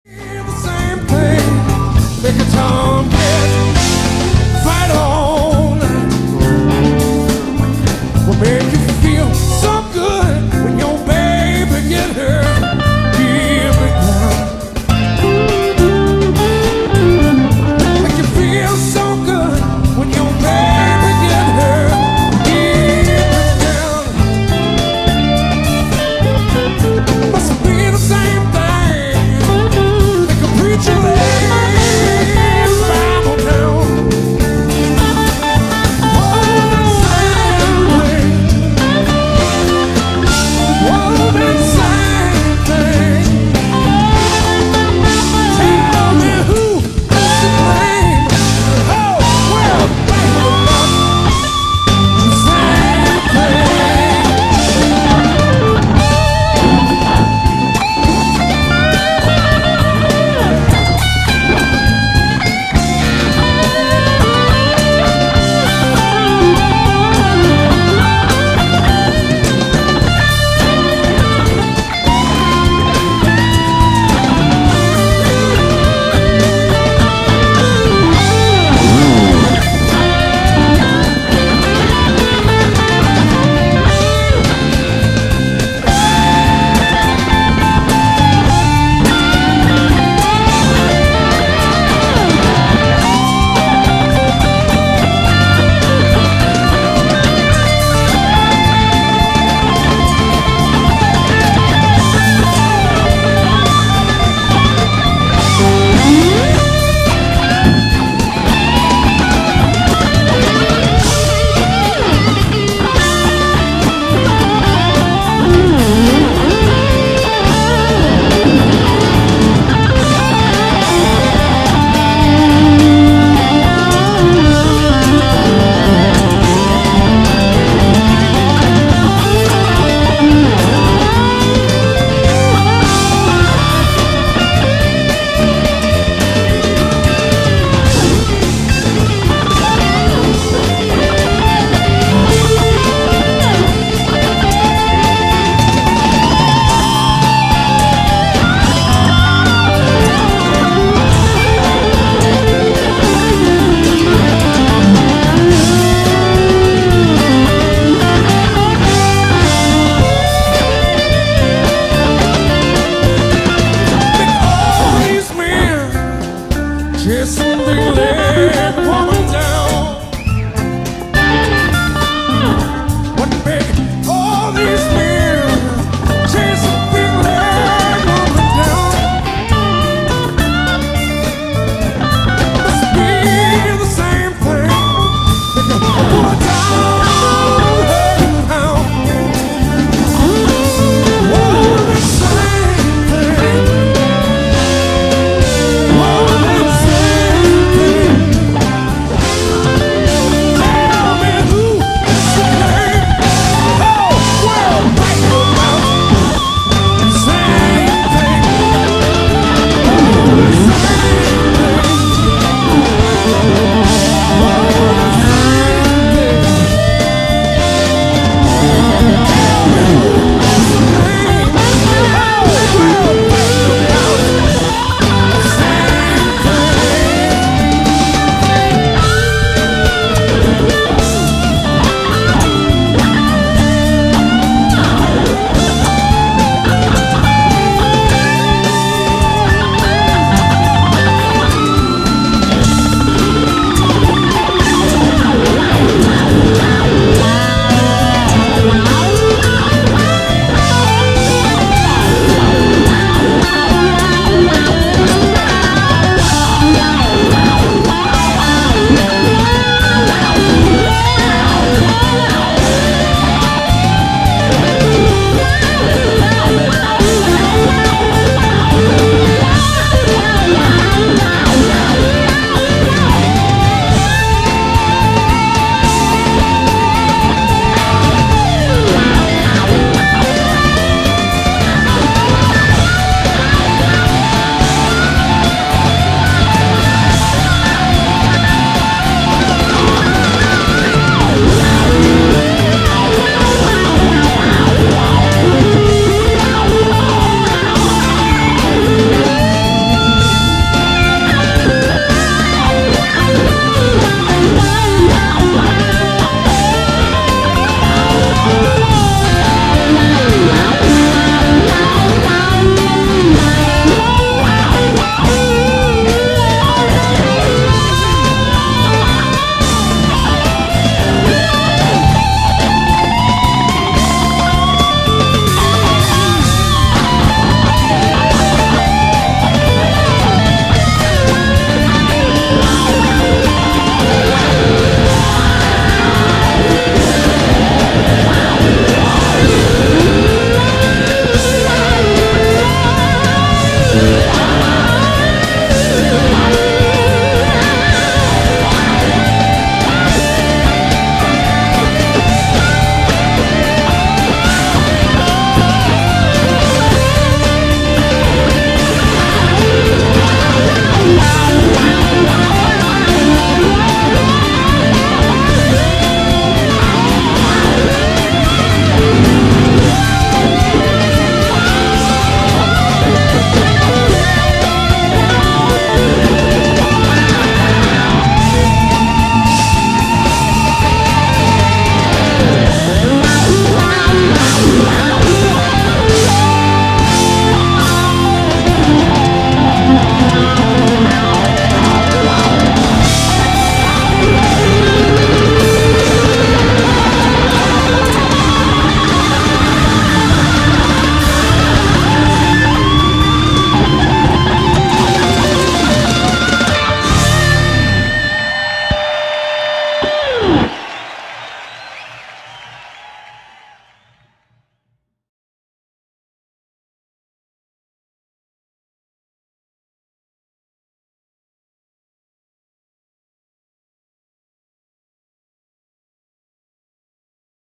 jammin with allman brothers live guitar jam
jammin along with allman brothers concert live from pandora, the recording sound great, i almost thought i was really playing with them for reals! dont know the title of the song  pleas let me know if you know it.  peace love joy!
fly on brother!